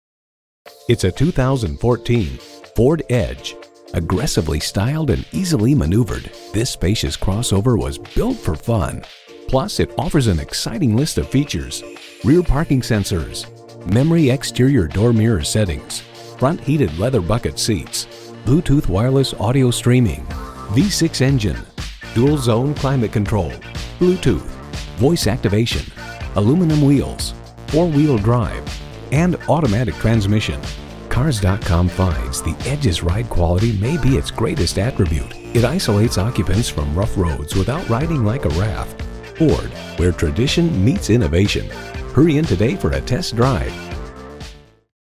Used 2014 Ford Edge Rochester MN Winona, MN JA1324 - SOLD
Year: 2014
Make: Ford
Model: Edge
Trim: Limited
Engine: 3.5L V6 Ti-VCT
Transmission: 6-Speed Automatic with Select-Shift
Color: Gray
Interior: Medium Light Stone
Mileage: 134452
Stock #: JA1324
VIN: 2FMDK4KC7EBA54262
Recent Arrival! Meets our Rigorous Inspection and Reconditioning Standards! Edge Limited Mineral Gray Metallic Clean CARFAX. This Ford Edge has many features and is well equipped including.brbrbrAwards:br * JD Power Initial Quality Study (IQS) * 2014 KBB.com Brand Image AwardsbrbrStop by, call or email us today at Rochester Car Clearance Center. We look forward to earning your business!

Address:
4900 Highway 52 North